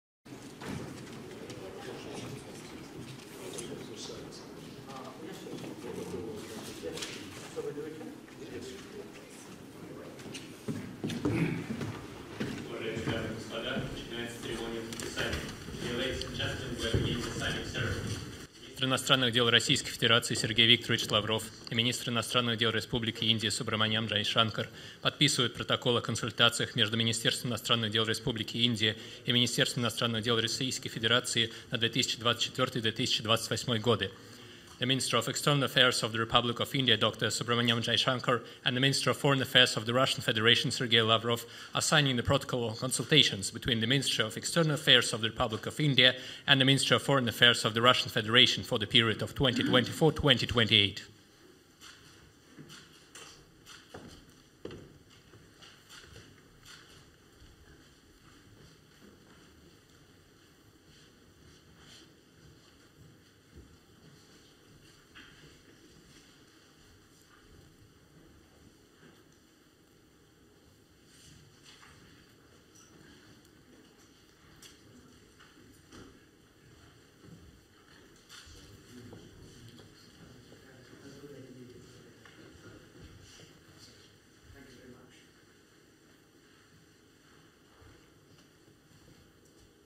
Церемония подписания протокола о консультациях между МИД России и МИД Индии на 2024-2028 гг.
Министр иностранных дел Российской Федерации С.В.Лавров и Министр иностранных дел Республики Индии С.Джайшанкар подписывают протокол о консультациях между МИД Российской Федерации и МИД Республики Индии на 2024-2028 гг., Москва, 27 декабря 2023 года